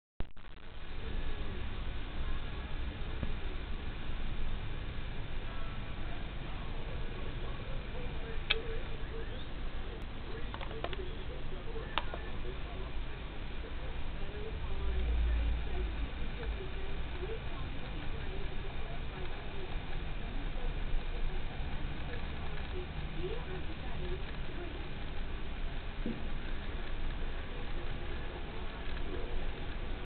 TCS Back EMF Shunting
The precise slow speed control with the loco moving off at Speed Step 1 every time, makes Back EMF decoders, the only choice if shunting is a prority on your layout.